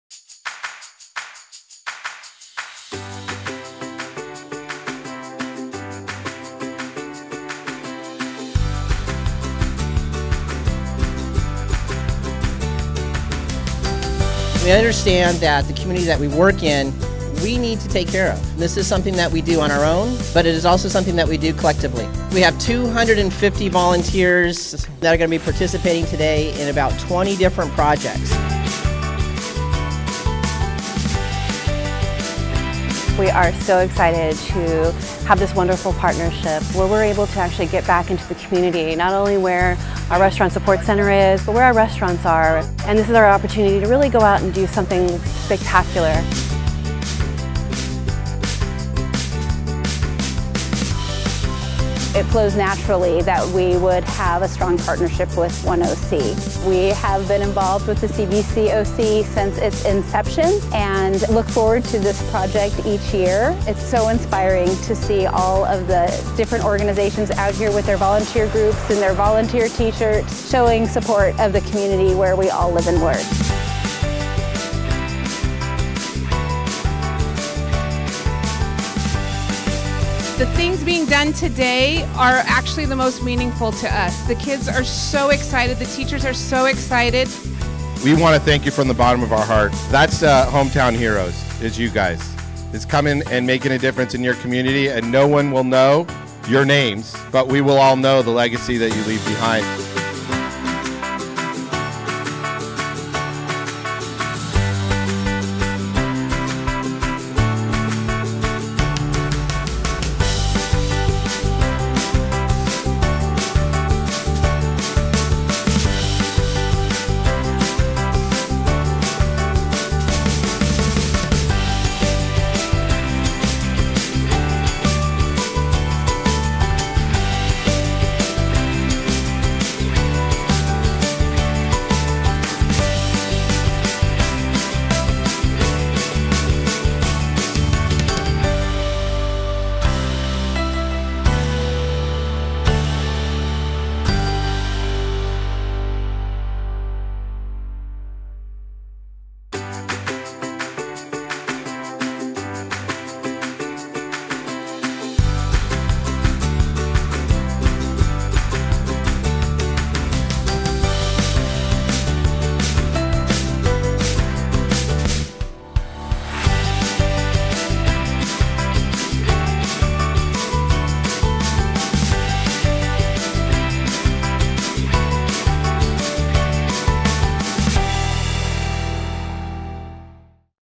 2017 CVCOC Project Highlight reel
On May 20, 2017 members of the CVCOC contributed to community pride and ownership as they joined together to serve Thorman Elementary and Currie Middle Schools in Tustin, CA.